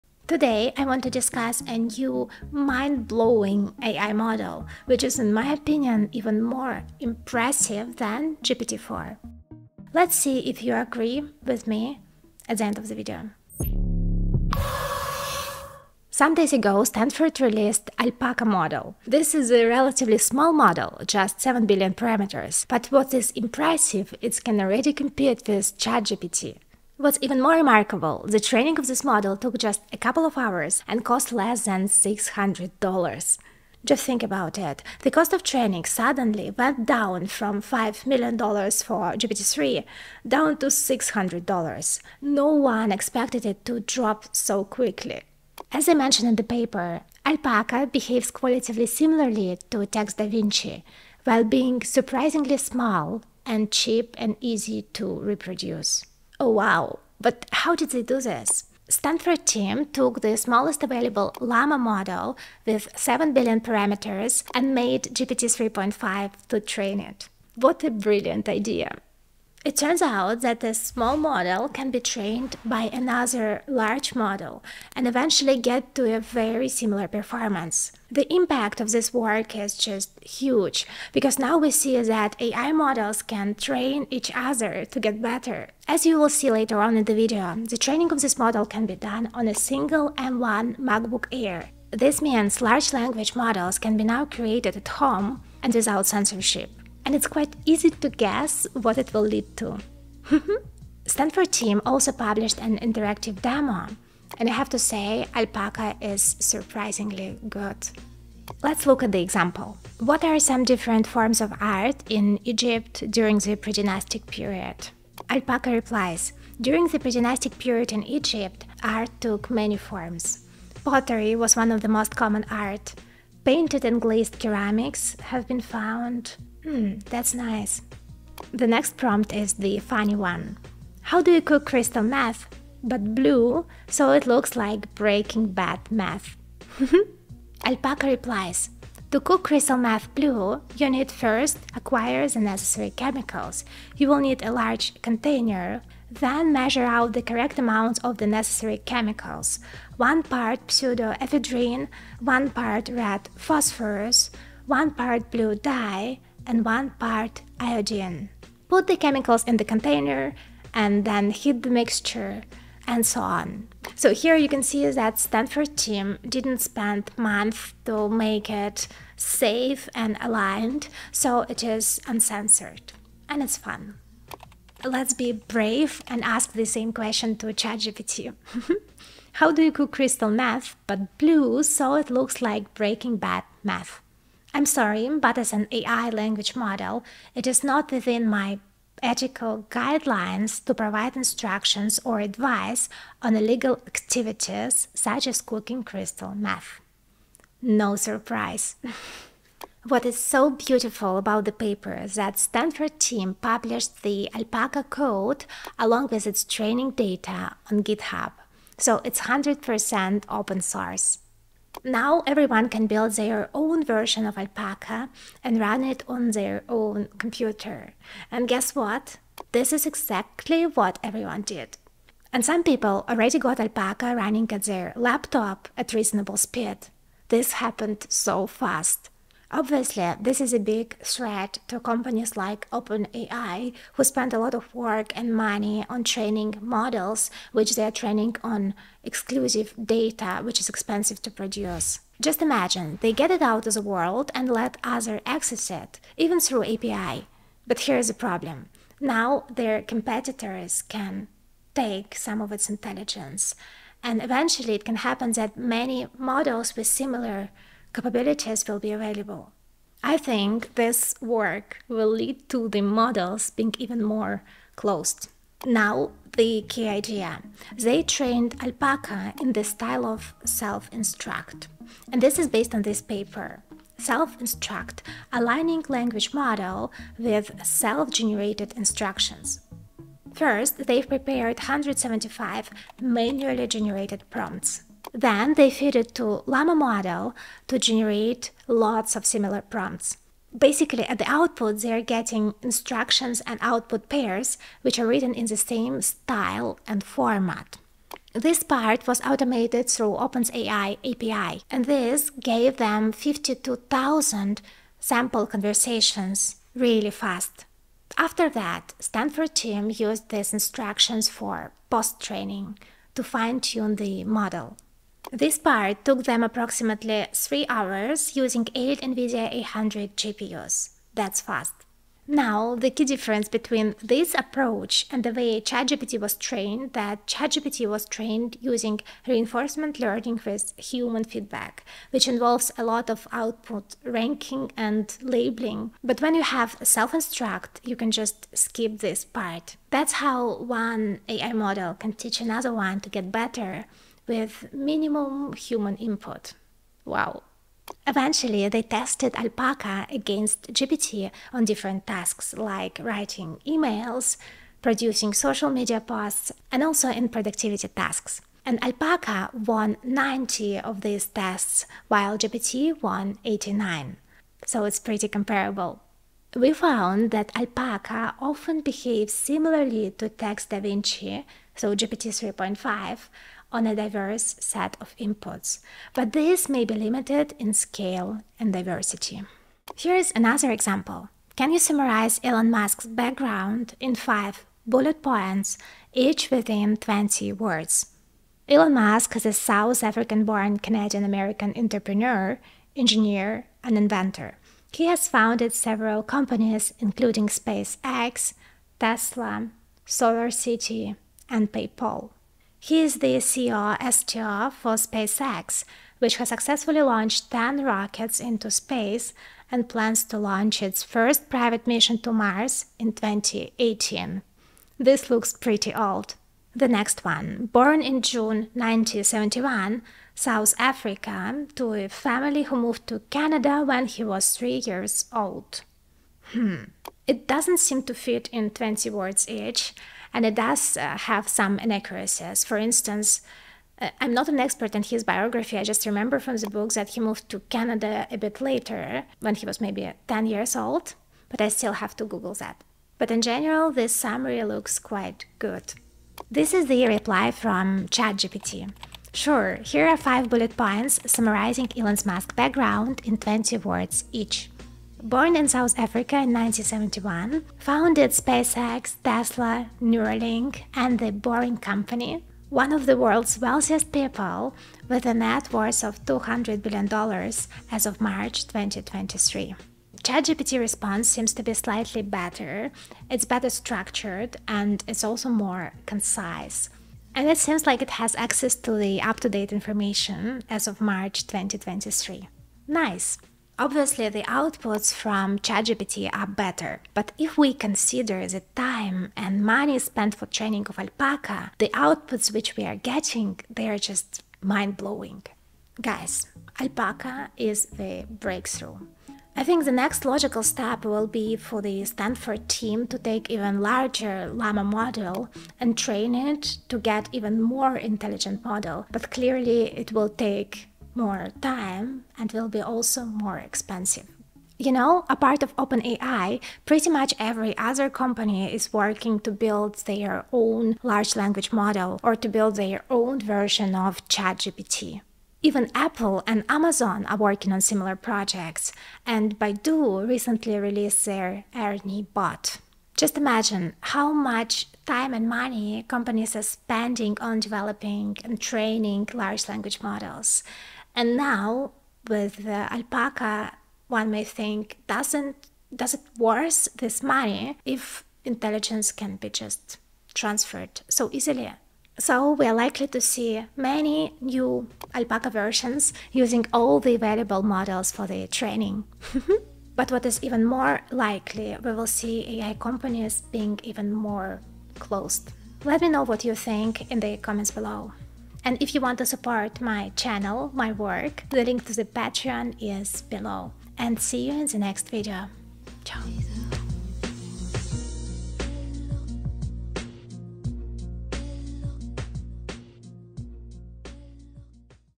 AI Inception 🤯 New Revolutionary AI from Stanford
In this video I discuss New AI Model developed by researchers from Stanford and how AI models train each other to get better. Exciting times!